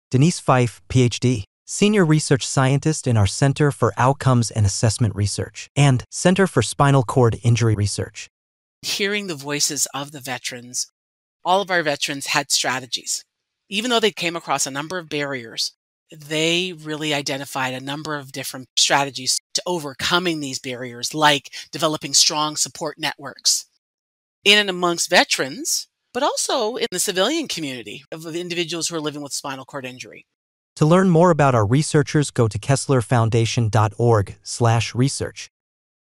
¡Feliz Día de los Veteranos 2023!
No todos los veteranos con lesión medular
reciben la misma asistencia sanitaria y prestaciones, lo
que provoca disparidades sanitarias y diversos
resultados en esta población. Para determinar las
causas de estas disparidades, los investigadores de la
Fundación Kessler estudian datos sobre edad,
duración de la lesión, tipo de lesión (relacionada o no
con el servicio) y tipo de prestaciones por discapacidad
y asistencia sanitaria.

ch